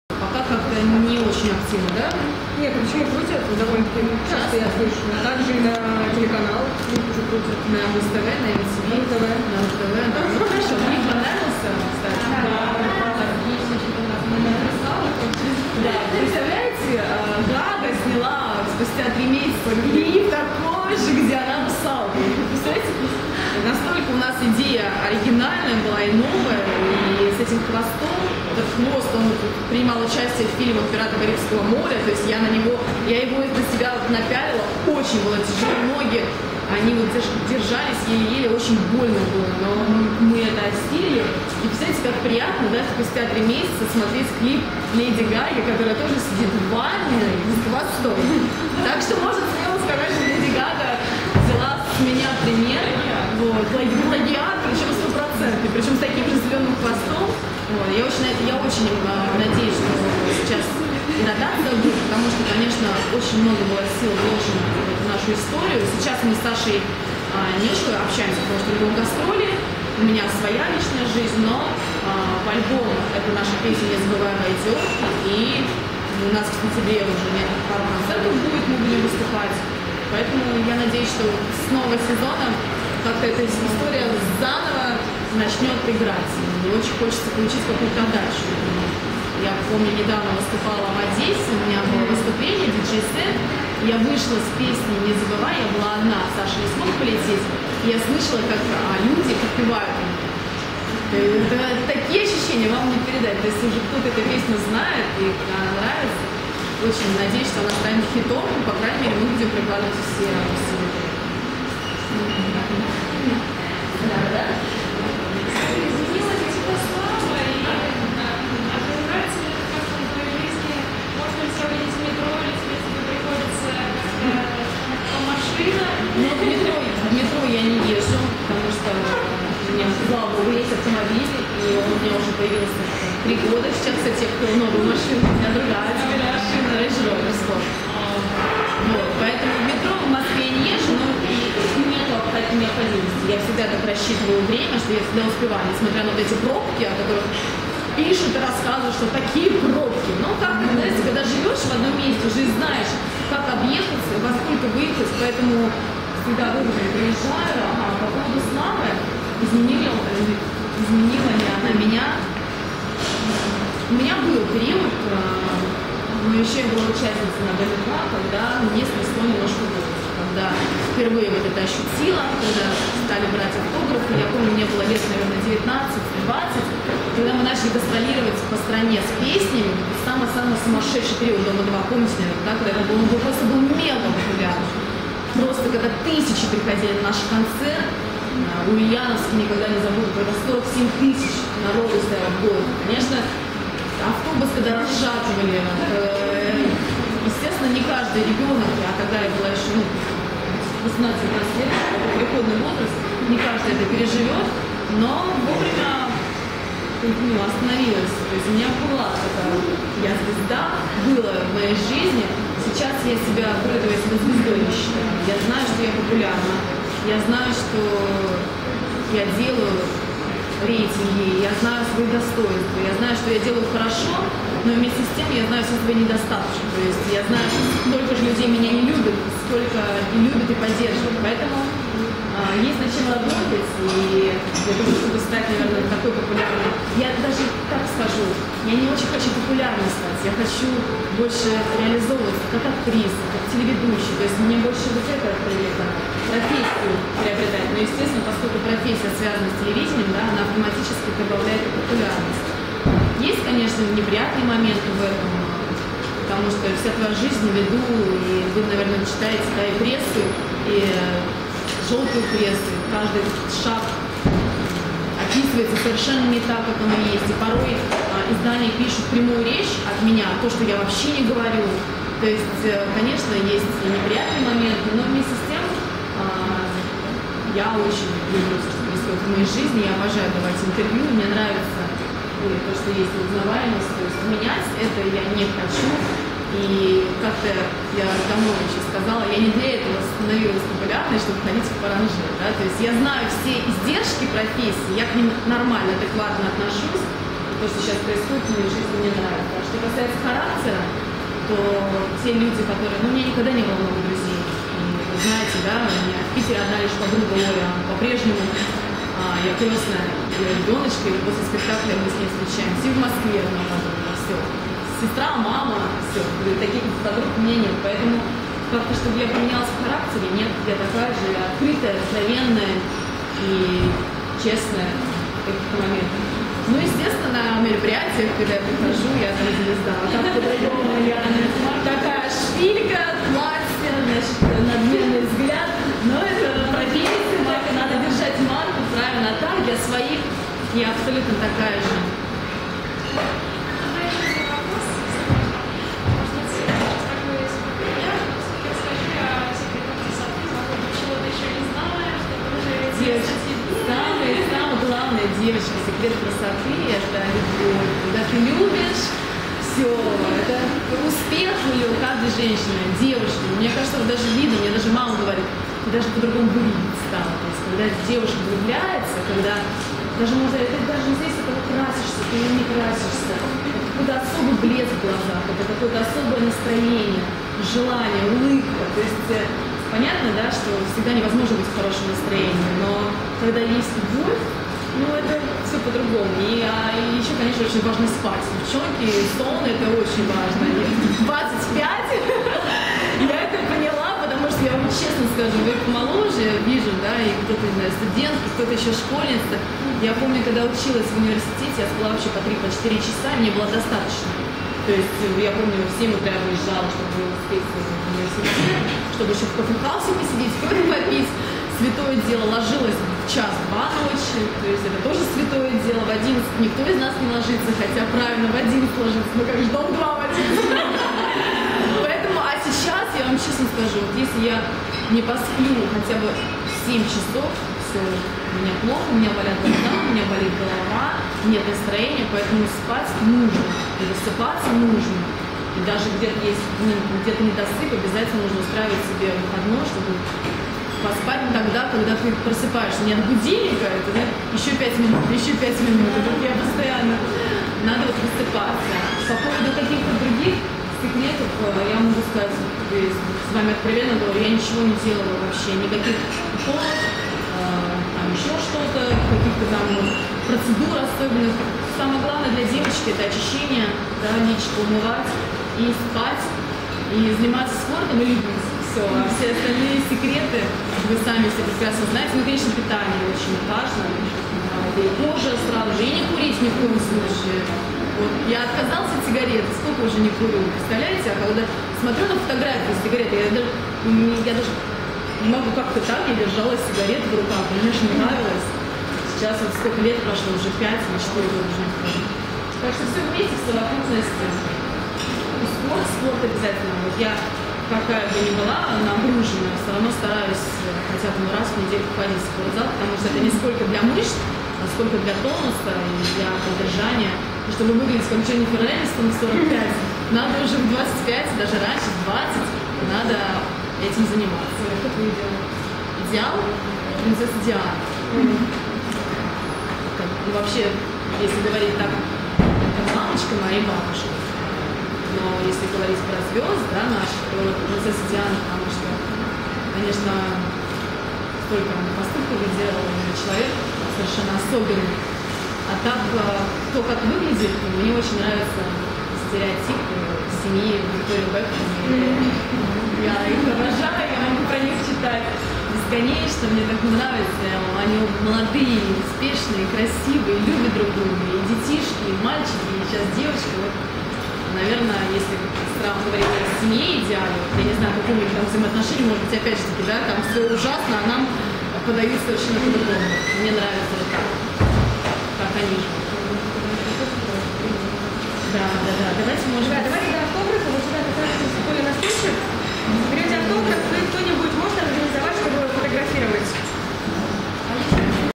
Ольга Бузова. Встреча с фанатами
23 августа в «Кофе-Хаусе» на Малой Конюшенной состоялась встреча популярной телеведущей Ольги Бузовой со своими поклонниками.